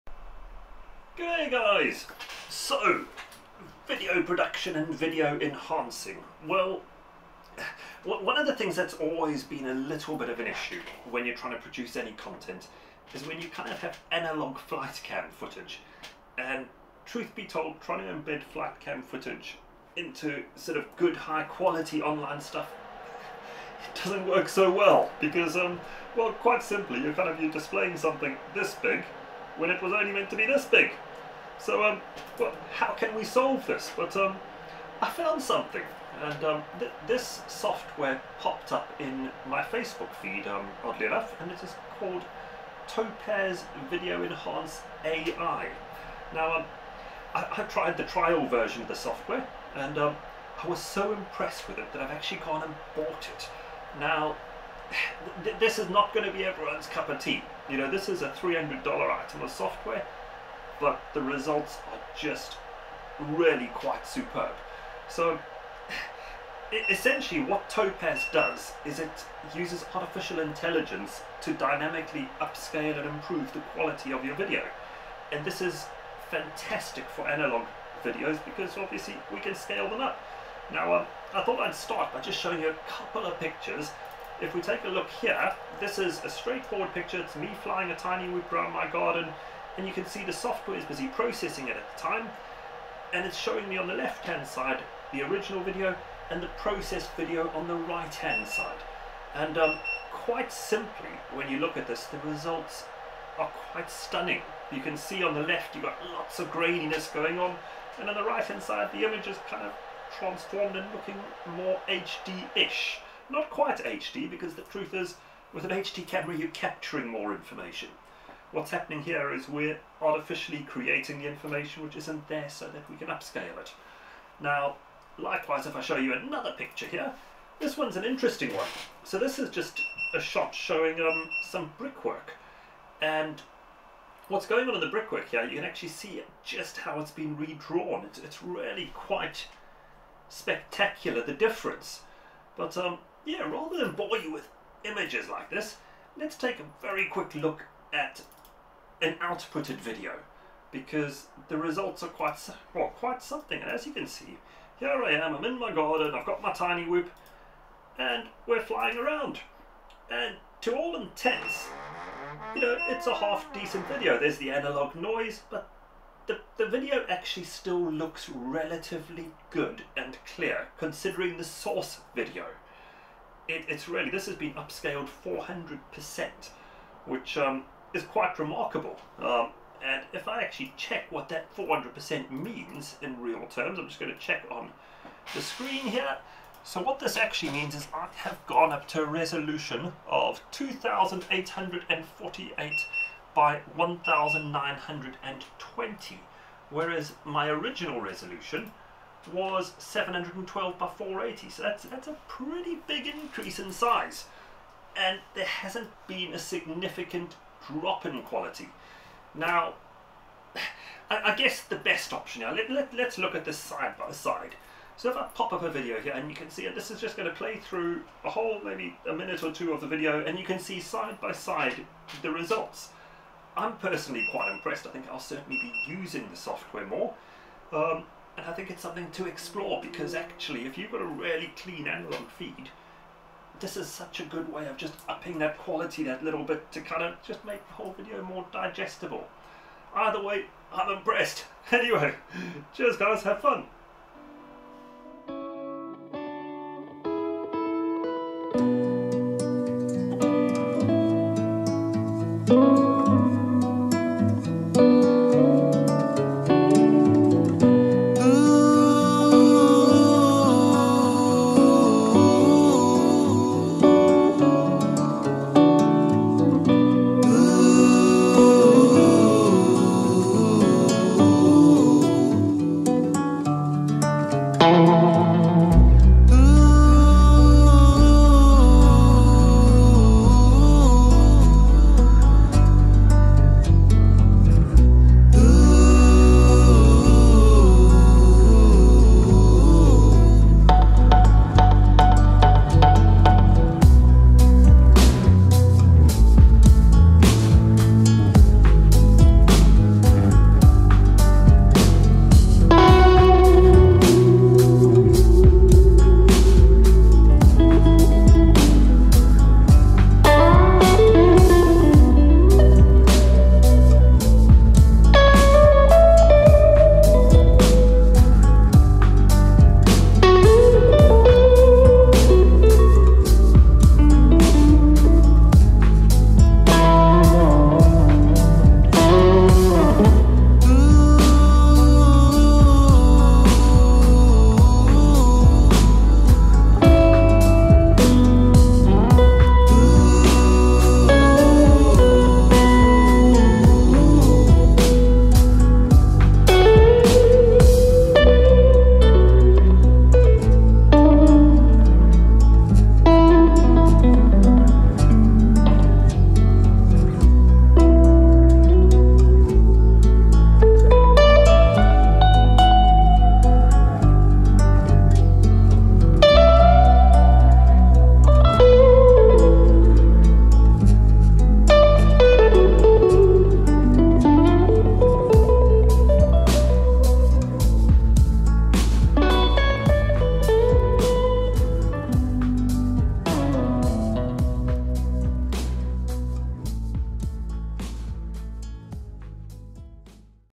WHAT!!... I CAN MAKE ANALOG DVR LOOK LIKE HD? | TOPAZ VIDEO ENHANCE AI TO THE RESCUE
I discovered some utterly fantastic software. It uses AI tech to upscale your video from SD to HD.

The result is that your analog dvr footage can be 'upscaled' to full hd - with near no loss of quality.

Happy flying...
LIKE THESE VIDEOS?  Consider leaving a tip...